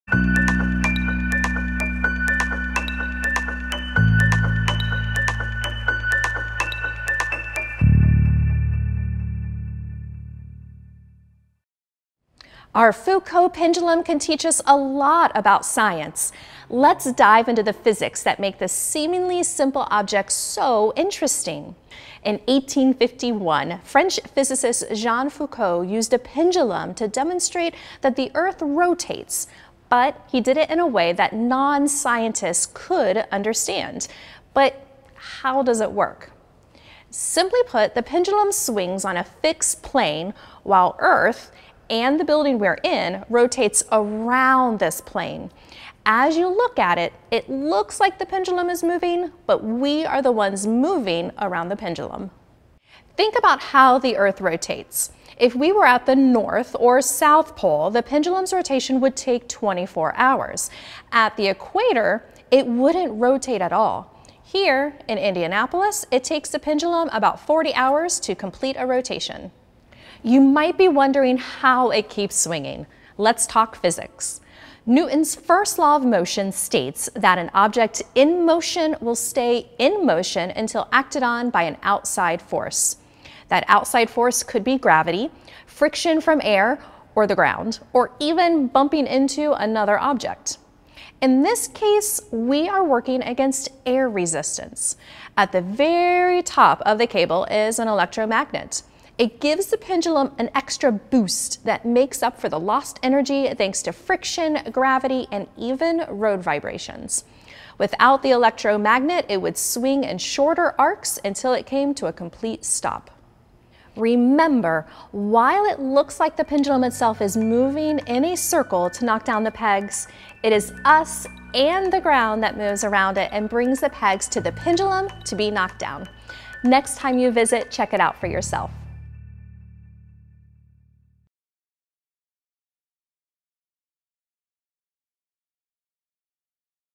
Pendulum
What can we learn from a pendulum? Come explore earth’s rotation, cardinal signs and force and motion as we investigate this amazing Foucault pendulum.